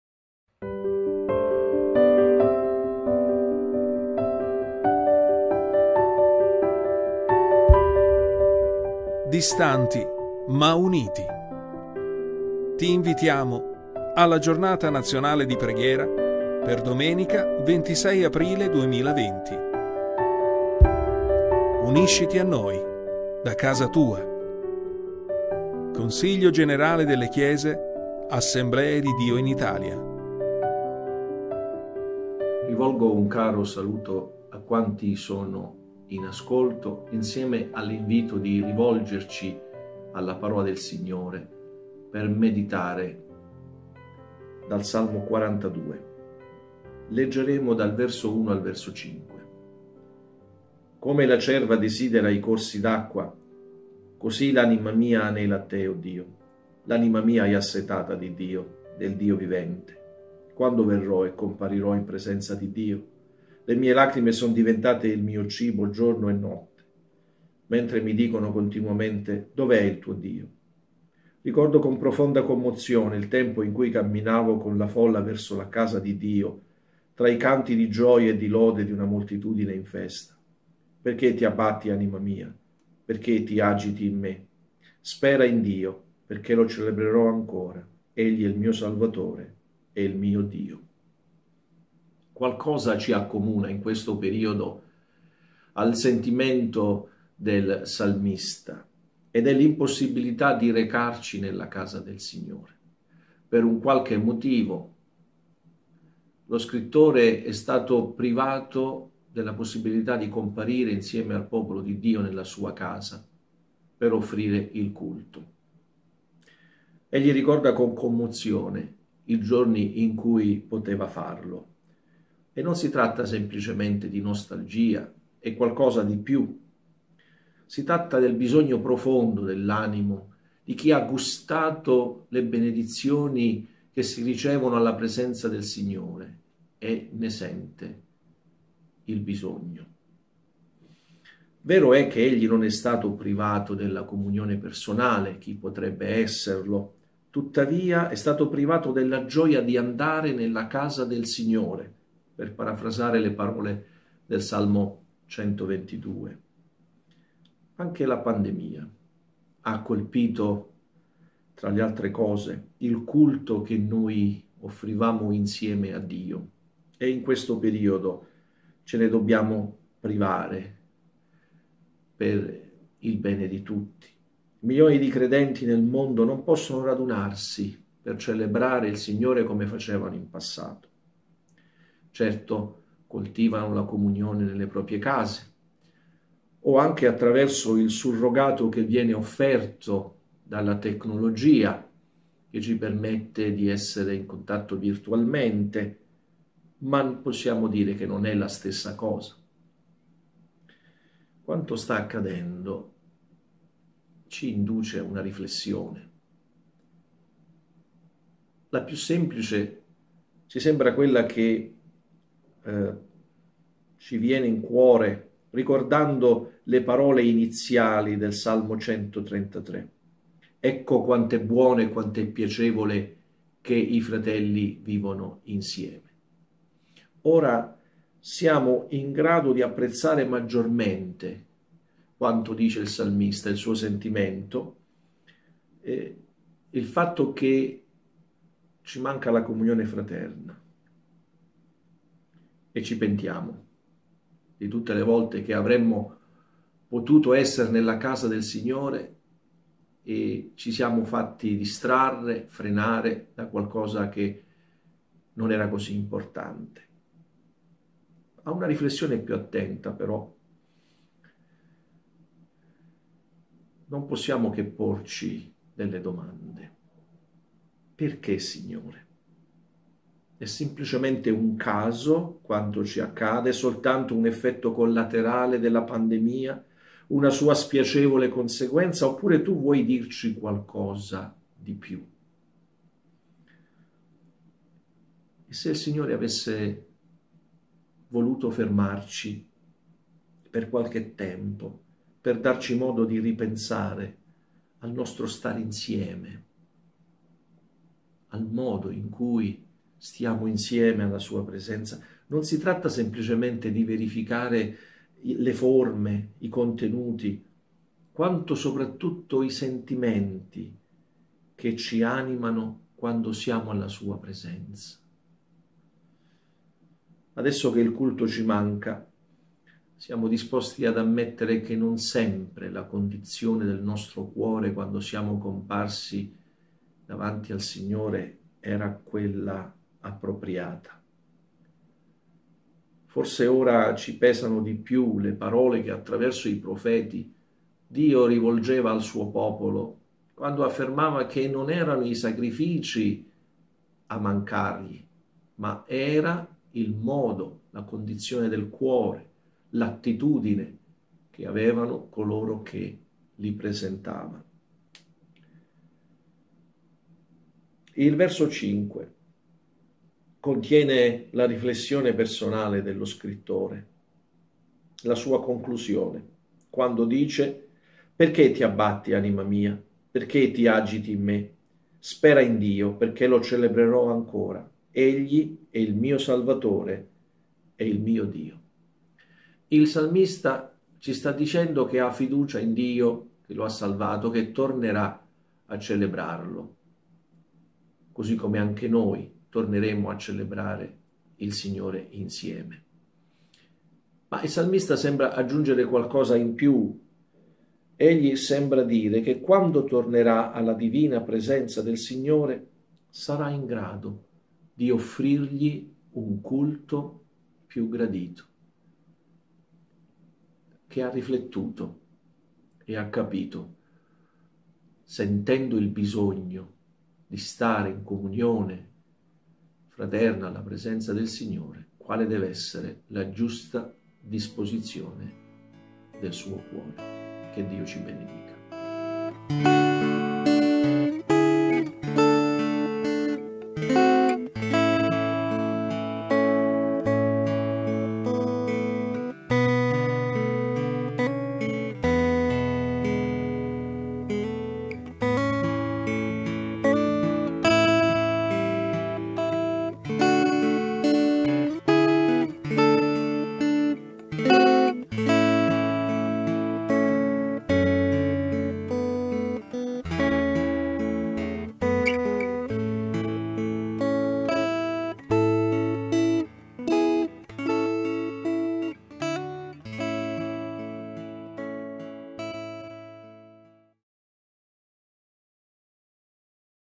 Giornata Nazionale di Preghiera - Ore 16:00
Ore 16:00 - La nostra Giornata Nazionale di Preghiera continua con il nono argomento, presentato dal Past. Vincenzo Martucci